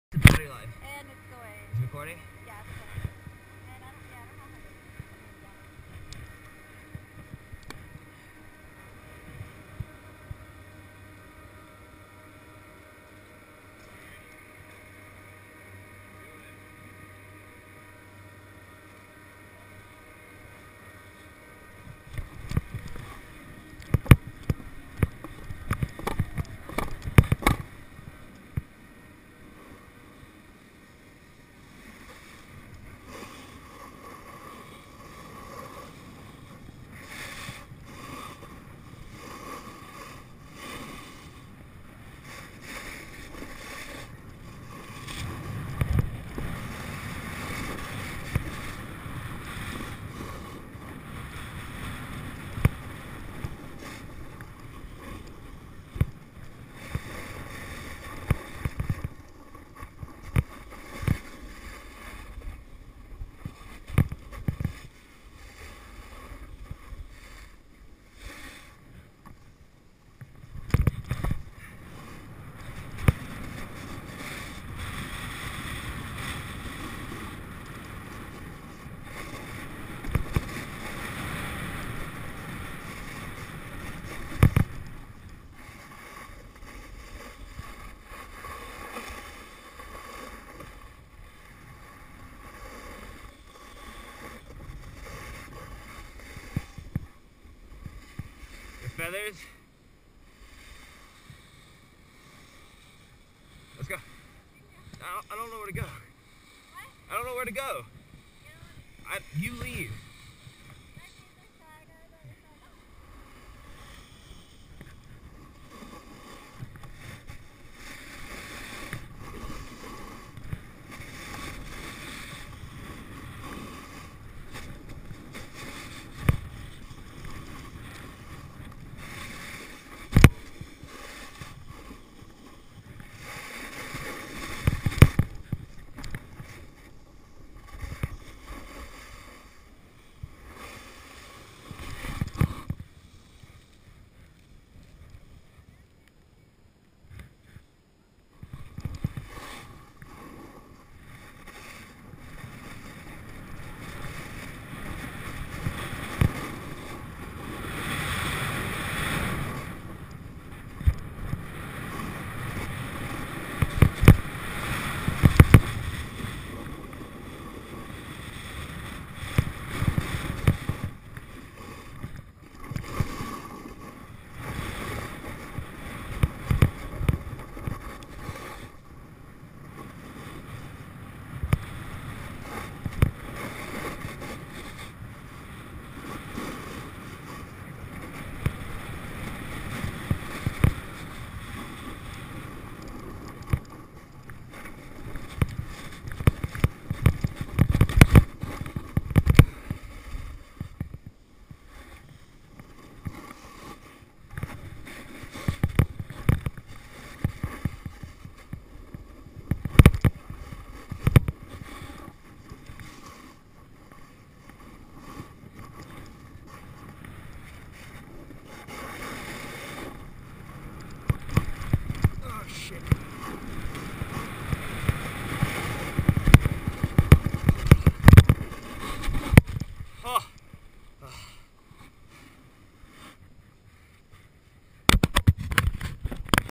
2012 spring beaver creek (Puno)
Julie and I doing some spring boarding at Beaver Creek, CO.